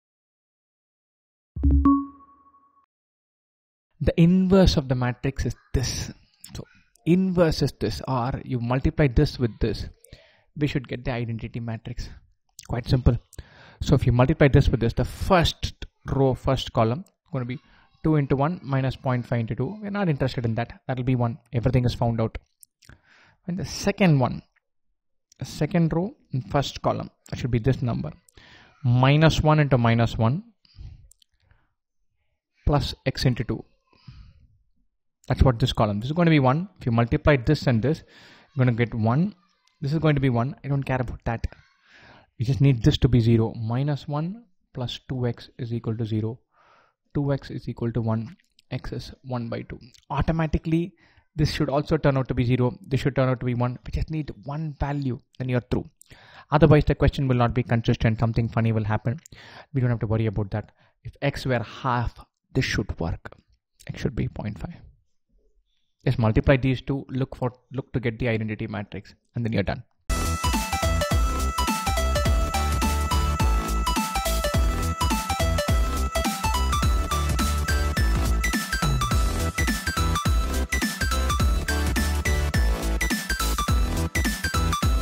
IPMAT 2019 Solutions | Previous Year Actual Paper -Matrices | IPM | IPMAT 2020 | CAT 2020
If the inverse of the matrix [-1      x] is  [2  4] then the value of x is?
a) 0.5
b) 1
c) 2
d) 3